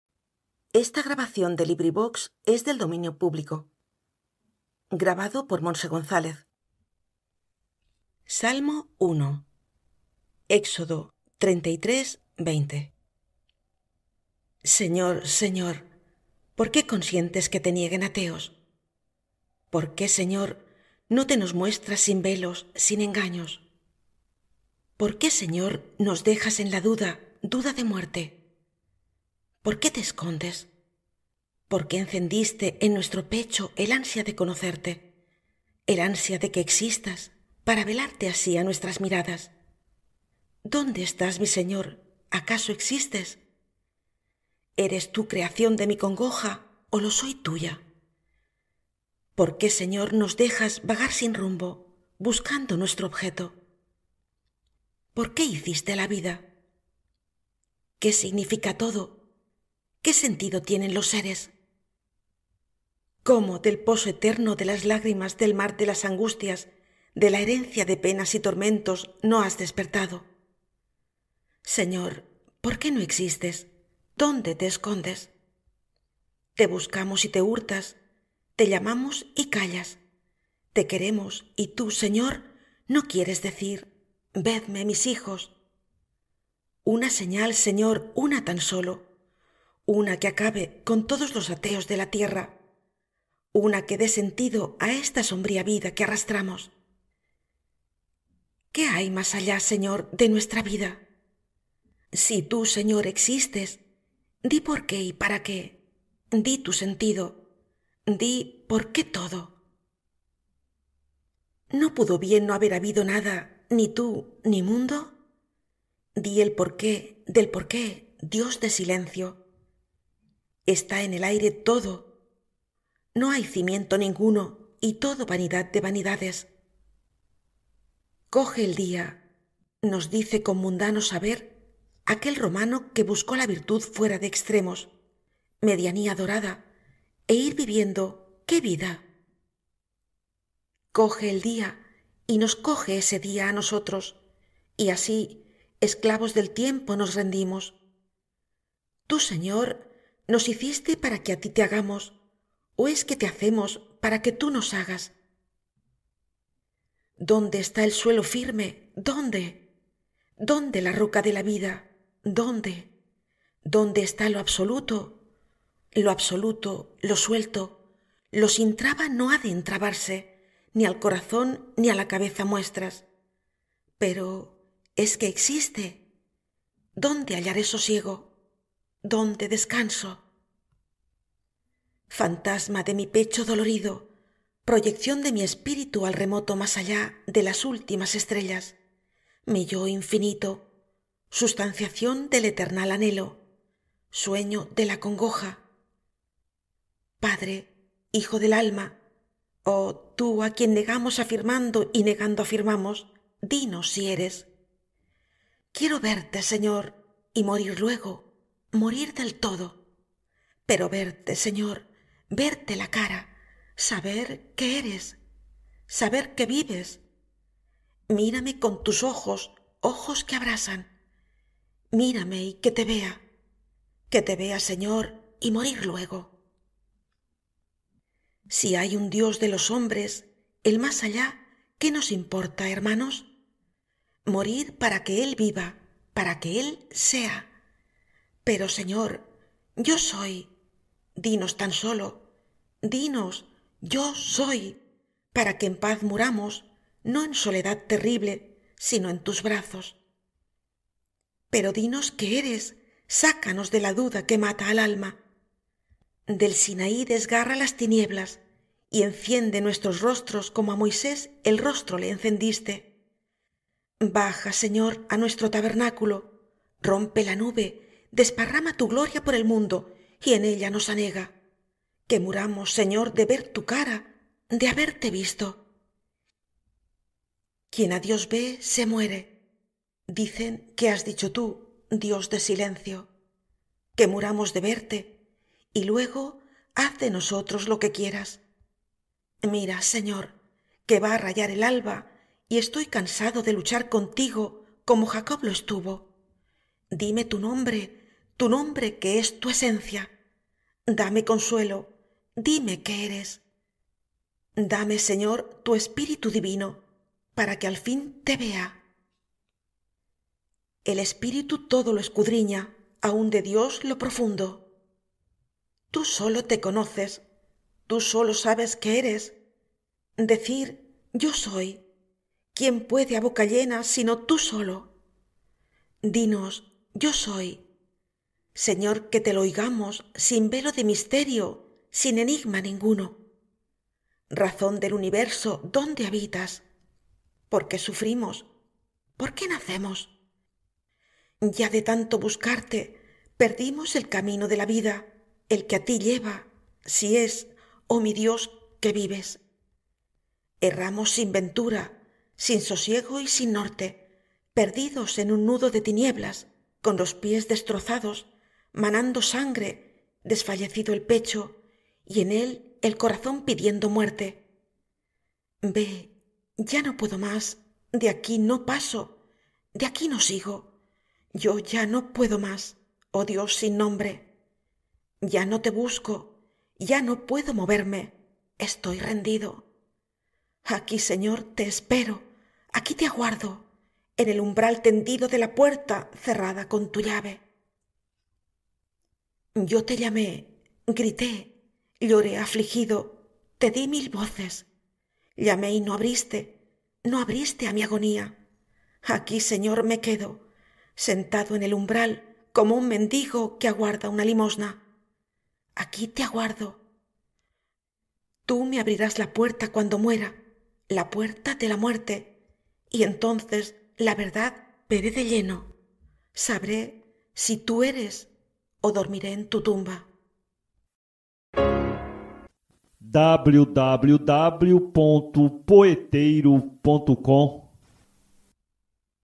"Salmo I" (Poema em Espanhol), de Miguel de Unamuno
SALMO I

Éxodo. XXXIII, 20.

Señor, Señor, ¿por qué consientes
que te nieguen ateos?
¿Por qué, Señor, no te nos muestras
sin velos, sin engaños?
¿Por qué, Señor, nos dejas en la duda,
duda de muerte?
¿Por qué te escondes?
¿Por qué encendiste en nuestro pecho el ansia
de conocerte,
el ansia de que existas,
para velarte así a nuestras miradas?
¿Dónde estás, mi Señor; acaso existes?
¿Eres Tú creación de mi congoja,
o lo soy tuya?
¿Por qué, Señor, nos dejas
vagar sin rumbo
buscando nuestro objeto?
¿Por qué hiciste la vida?
¿Qué significa todo, qué sentido
tienen los seres?

¿Cómo del poso eterno de las lágrimas,
del mar de las angustias,
de la herencia de penas y tormentos
no has despertado?
Señor, ¿por qué no existes?
¿dónde te escondes?
Te buscamos y te hurtas,
te llamamos y callas,
te queremos y Tú, Señor, no quieres
decir: ¡vedme, mis hijos!

Una señal. Señor, una tan sólo,
una que acabe
con todos los ateos de la tierra;
una que dé sentido
a esta sombría vida que arrastramos.

¿Qué hay más allá, Señor, de nuestra vida?
Si Tú, Señor, existes,
¡di por qué y para qué, di tu sentido!
¡di por qué todo!
¿No pudo bien no haber habido nada
ni Tú, ni mundo?

¡Di el por qué del por qué. Dios de silencio!
Está en el aire todo,
no hay cimiento ninguno
y todo vanidad de vanidades.

«¡Coge el día!», nos dice
con mundano saber aquel romano
que buscó la virtud fuera de extremos,
medianía dorada
e ir viviendo... ¿qué vida?
«¡Coge el día!» y nos coge
ese día a nosotros,
y así esclavos del tiempo nos rendimos.

¿Tú, Señor, nos hiciste
para que a Ti te hagamos,
o es que te hacemos
para que Tú nos hagas?

¿Dónde está el suelo firme, dónde?
¿Dónde la roca de la vida, dónde?
¿Dónde está lo absoluto?
¡Lo absoluto, lo suelto, lo sin traba
no ha de entrabarse
ni al corazón ni a la cabeza nuestras!

Pero... ¿es que existe?
¿Dónde hallaré sosiego?
¿dónde descanso?

¡Fantasma de mi pecho dolorido;
proyección de mi espíritu al remoto
más allá de las últimas estrellas;
mi yo infinito;
sustanciación del eternal anhelo;
sueño de la congoja;
Padre, Hijo del alma;
¡oh Tú, a quien negamos afirmando
y negando afirmamos
dinos si eres!

¡Quiero verte, Señor, y morir luego,
morir del todo;
pero verte, Señor, verte la cara,
saber que eres!
¡saber que vives!
¡Mírame con tus ojos,
ojos que abrasan;
mírame y que te vea!
¡que te vea, Señor, y morir luego!
Si hay un Dios de los hombres,
¿el más allá qué nos importa, hermanos?

¡Morir para que Él viva,
para que Él sea!
Pero, Señor, «¡yo soy!» dinos tan sólo,
dinos «yo soy» para que en paz muramos,
no en soledad terrible,
sino en tus brazos!
Pero dinos que eres,
¡sácanos de la duda
que mata al alma!
Del Sinaí desgarra las tinieblas
y enciende nuestros rostros
como a Moisés el rostro le encendiste;
baja, Señor, a nuestro tabernáculo,
rompe la nube,
desparrama tu gloria por el mundo
y en ella nos anega;
¡que muramos, Señor, de ver tu cara,
de haberte visto!

«Quien a Dios ve, se muere»
dicen que has dicho Tú, Dios de silencio;
¡que muramos de verte
y luego haz de nosotros lo que quieras!
¡Mira, Señor, que va a rayar el alba
y estoy cansado de luchar contigo
como Jacob lo estuvo!

¡Dime tu nombre!
¡tu nombre, que es tu esencia!
¡dame consuelo!
¡dime que eres!
¡Dame, Señor, tu espíritu divino,
para que al fin te vea!
El espíritu todo lo escudriña,
aun de Dios lo profundo.
Tú sólo te conoces,
Tú sólo sabes que eres.

Decir «¡yo soy!» ¿quién puede a boca llena
si no Tú sólo?
¡Dinos «¡yo soy!». Señor, que te lo oigamos,
sin velo de misterio
sin enigma ninguno!

Razón de Universo, ¿dónde habitas?
¿por qué sufrimos?
¿por qué nacemos?
Ya de tanto buscarte
perdimos el camino de la vida,
el que a Ti lleva
si es, ¡oh mi Dios!, que vives.
Erramos sin ventura,
sin sosiego y sin norte,
perdidos en un nudo de tinieblas,
con los pies destrozados,
manando sangre,
desfallecido el pecho,
y en él el corazón pidiendo muerte.
Ve, ya no puedo más, de aquí no paso,
de aquí no sigo,
aquí me quedo,
yo ya no puedo más, ¡oh Dios sin nombre!
Ya no te busco,
ya no puedo moverme, estoy rendido;
aquí, Señor, te espero,
aquí te aguardo,
en el umbral tendido de la puerta
cerrada con tu llave.

Yo te llamé, grité, lloré afligido,
te di mil voces;
llamé y no abriste,
no abriste a mi agonía;
aquí, Señor, me quedo,
sentado en el umbral como un mendigo
que aguarda una limosna;
aquí te aguardo.
Tú me abrirás la puerta cuando muera,
la puerta de la muerte,
y entonces la verdad veré de lleno,
sabré si Tú eres
o dormiré en tu tumba.